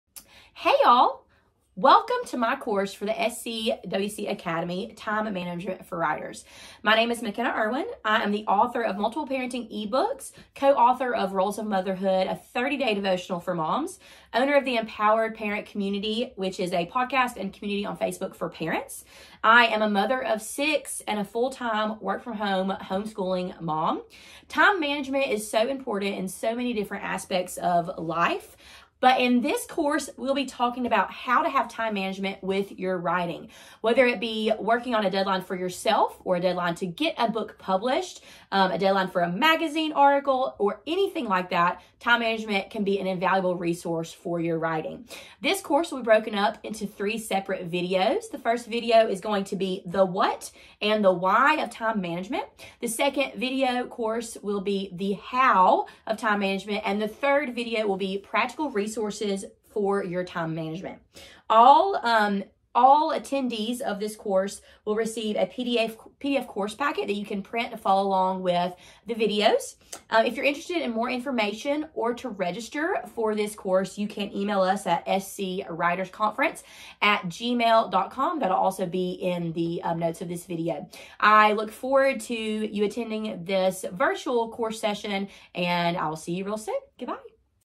“Time Management for Writers” Course Trailer
Join McKenna for a course on time management, the importance of effectively using your time for your writing , and practical resources to use for your writing. This course is $29 and includes 3 sessions plus a PDF course guide.